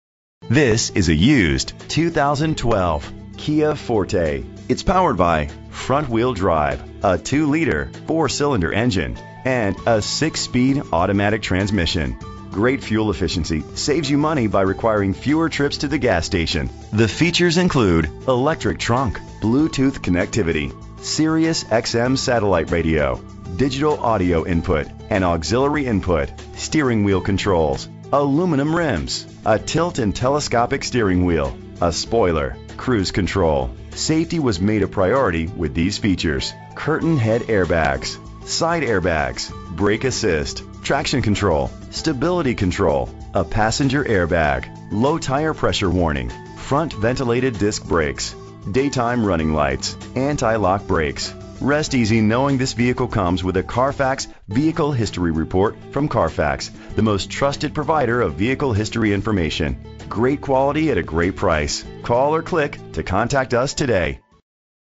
2012 Kia Forte - Oak Lawn IL
Mancari's of Oak Lawn

Mancari's Chrysler Jeep Dodge Ram is your premier auto dealer in Oak Lawn Illinois

Year: 2012
Make: Kia
Model: Forte
Trim: EX
Engine: 2.0 liter inline 4 cylinder 16 valve
Transmission: 6-Speed Automatic
Color: Ebony Black Pearl
Mileage: 38094

Address:
4630 West 95th St.